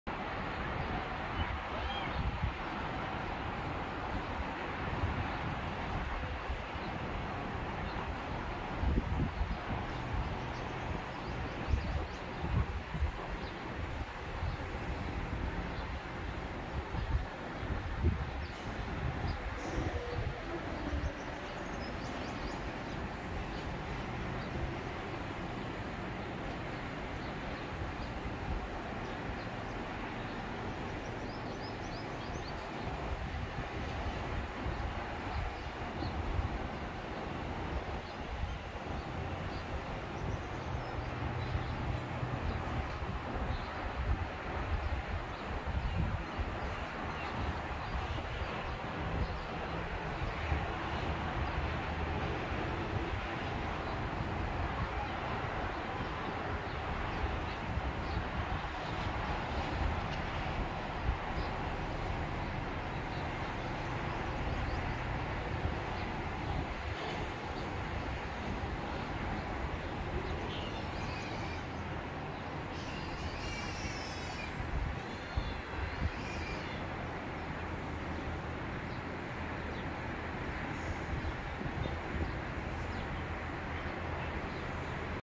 Beautiful Corniche Beach Abu Dhabi UAE 🇦🇪 ♥️
Enjoy full sea views from apartment balcony with the stunning corniche beach and corniche road awaits your intense presence.